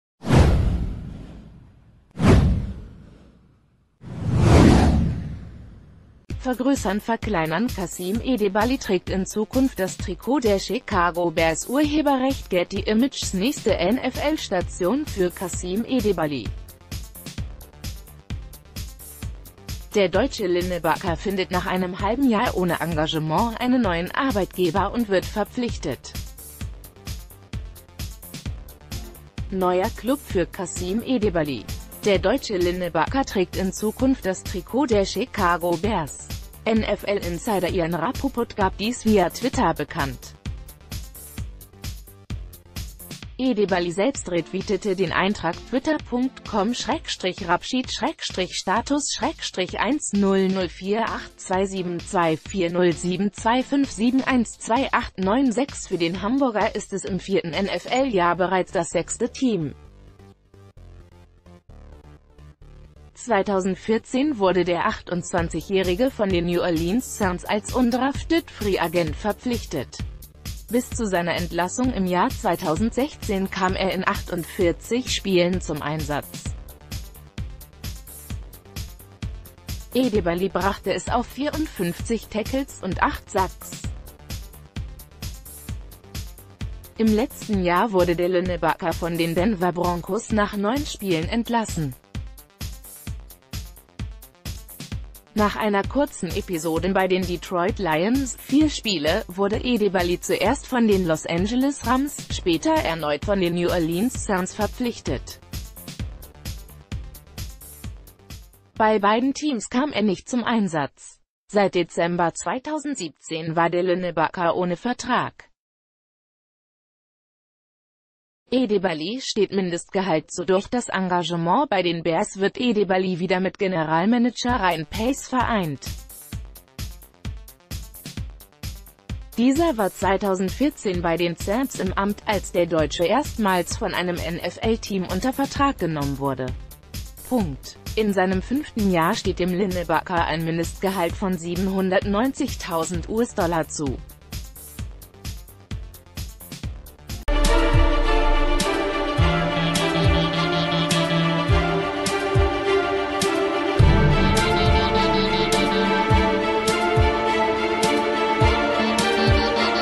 NFL: Chicago Bears nehmen Kasim Edebali unter Vertrag
vergrößernverkleinern Kasim Edebali trägt in Zukunft das Trikot der Chicago Bears © Getty Images Nächste NFL-Station für Kasim Edebali. Der deutsche Linebacker findet nach einem halben Jahr ohne Engagement einen neuen Arbeitgeber und wird verpflichtet. Neuer Klub für Kasim Edebali. Der deutsche Linebacker trägt in Zukunft das Trikot der Chicago Bears. NFL-Insider Ian Rapoport gab dies via Twitter bekannt. Edebali selbst retweetete den Eintrag// d...